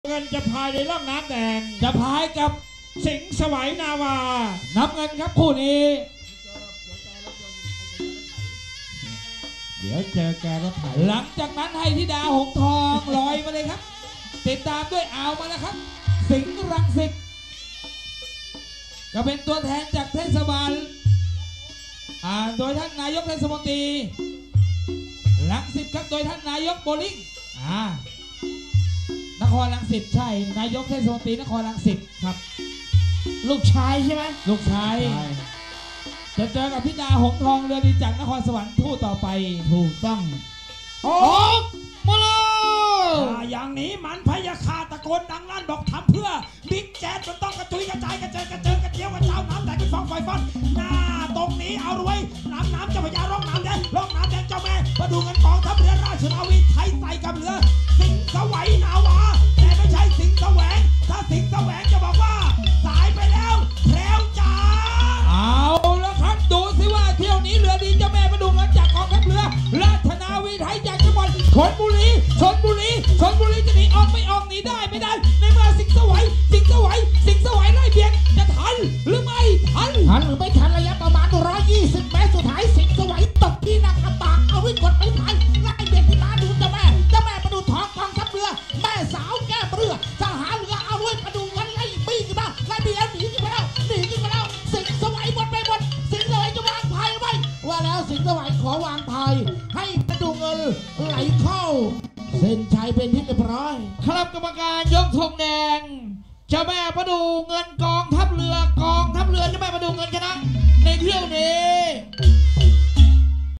เจ้าแม่ประดู่เงิน vs สิงห์ไสวนาวา 40 ฝีพาย เที่ยว 2 สนามสวนเทพปทุม 4 ธ.ค. 2565
แข่งเรือปทุมธานี ณ สวนเทพปทุม จ.ปทุมธานี 4 ธ.ค. 2565
Cr.ไทยลองโบท TLB